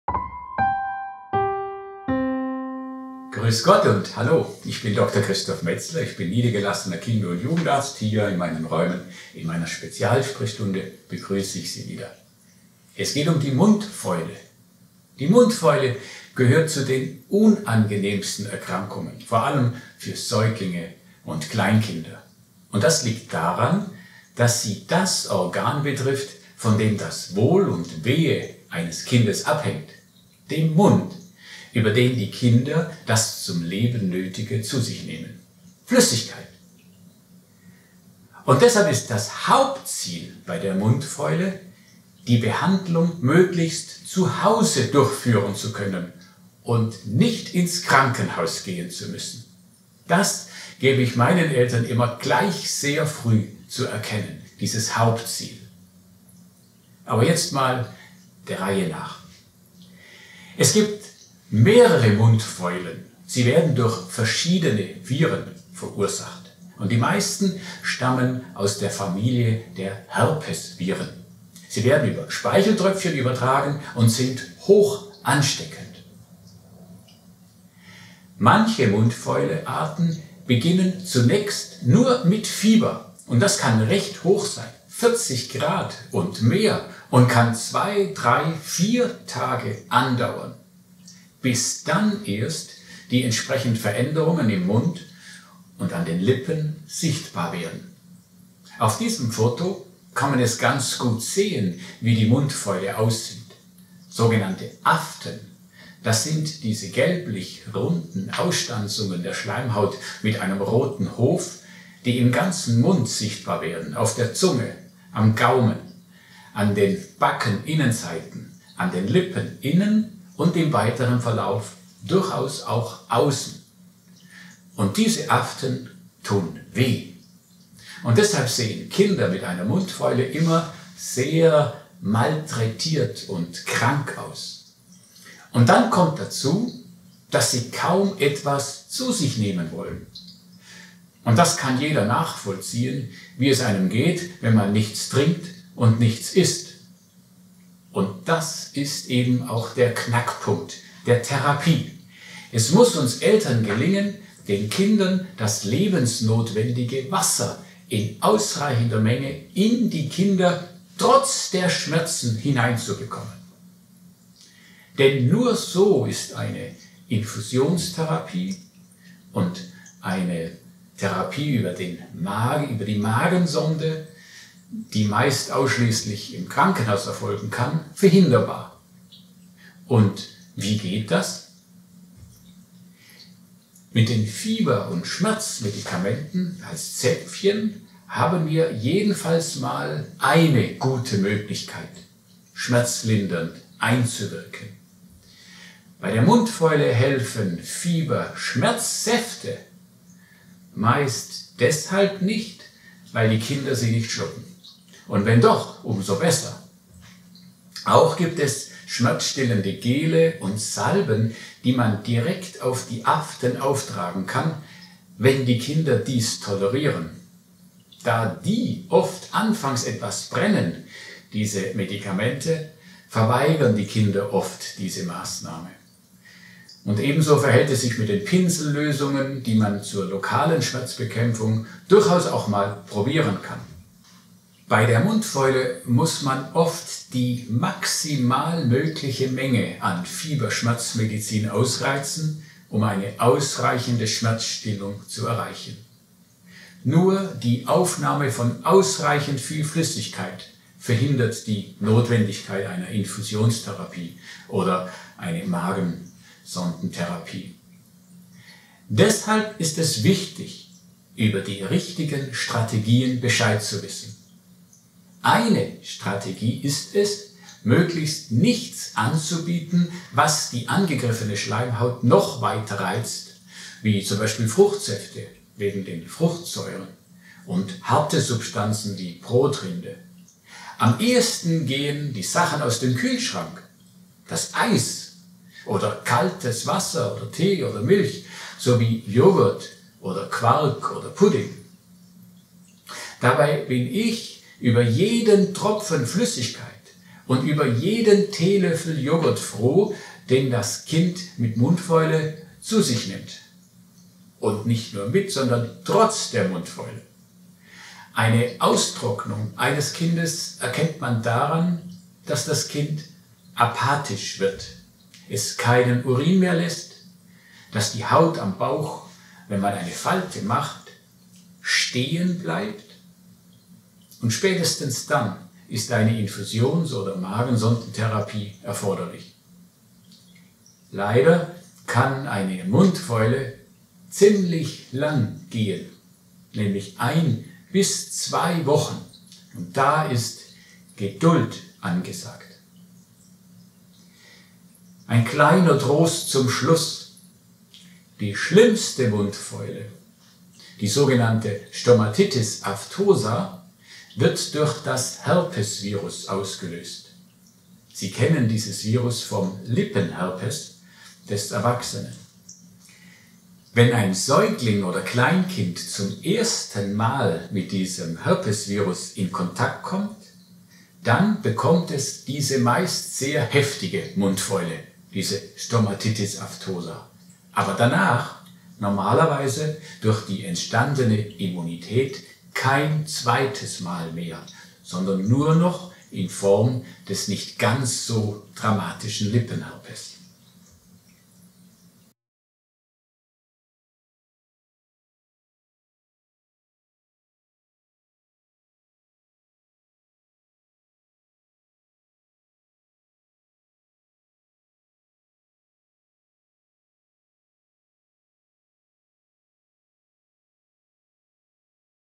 Mundfäule - Der Kinderarzt vom Bodensee
Aus seiner persönlichen und seiner Sprechstundenerfahrung heraus spricht der Kinder- und Jugendarzt Dr. med. Christof Metzler über die Mundfäule – wie man sie erkennt, worauf es bei der Behandlung ankommt und was Sie den Kindern am besten zu Essen geben.